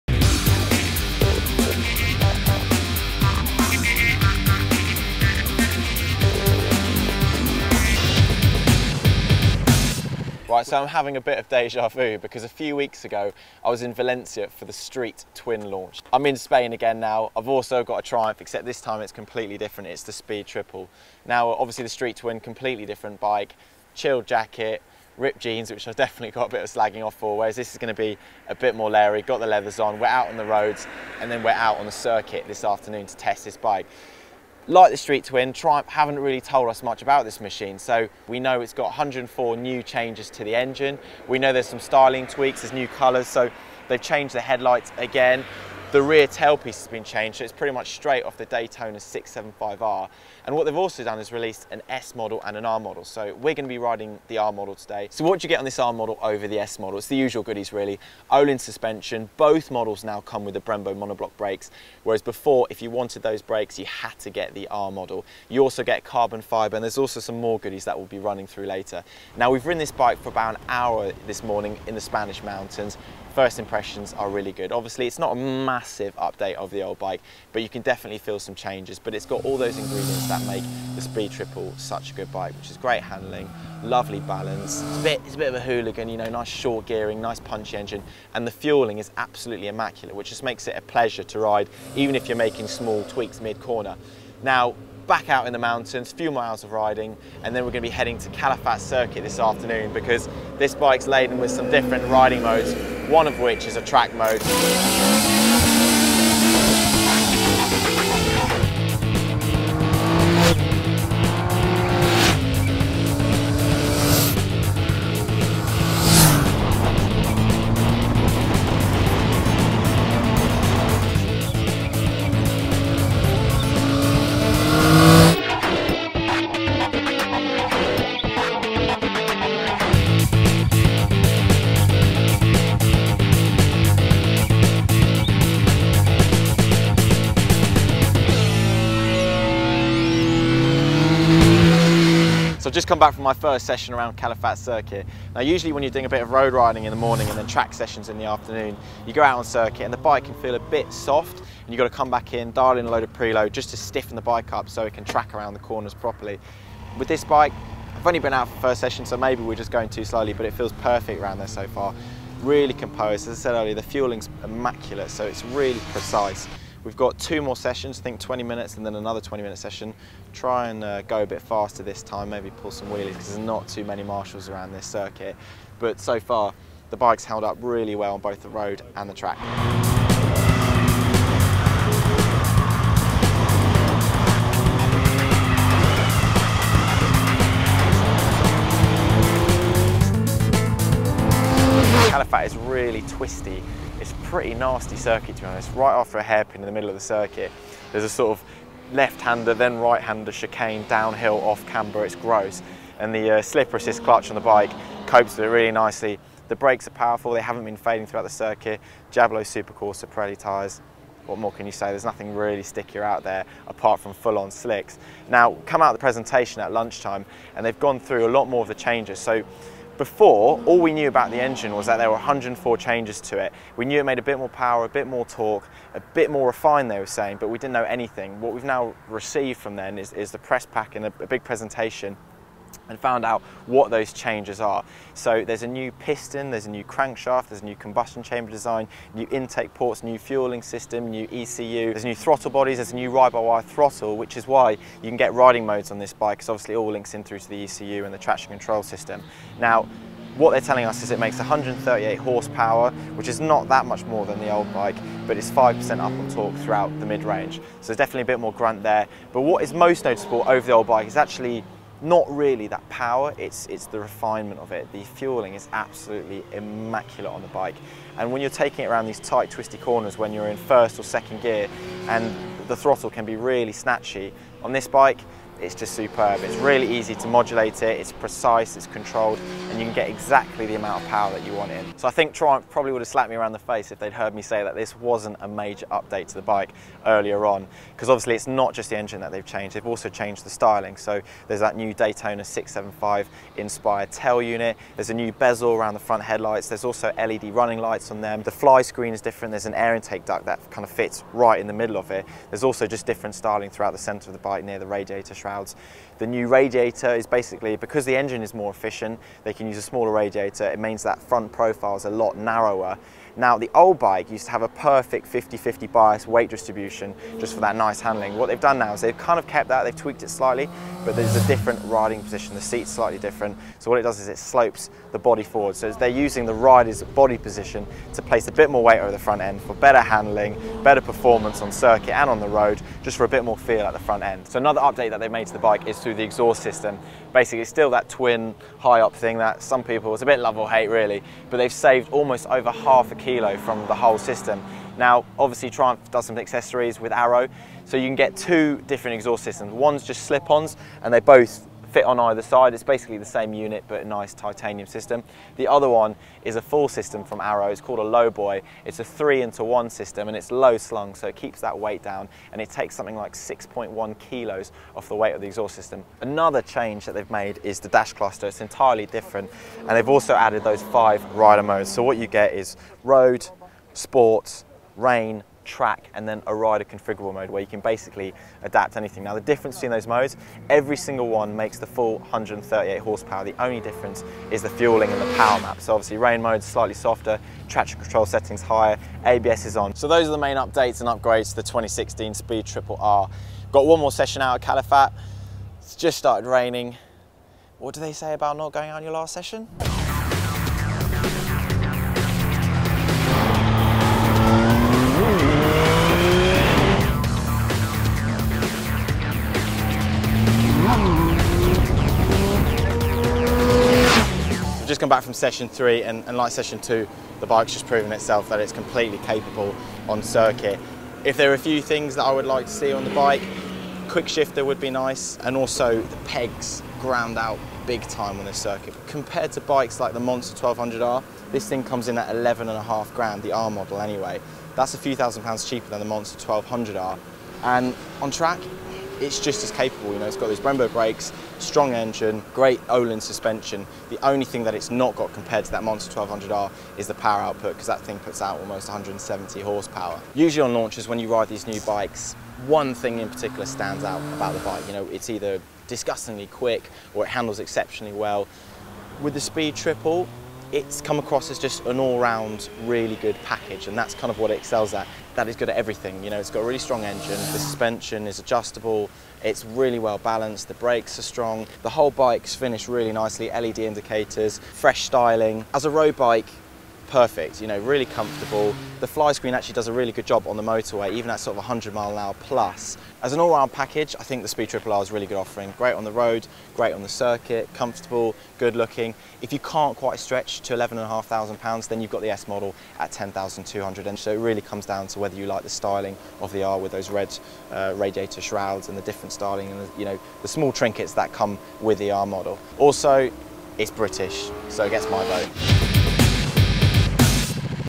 2016 Triumph Speed Triple Review (First Ride)
We're back in Spain again, this time for the launch of the new 2016 Triumph Speed Triple.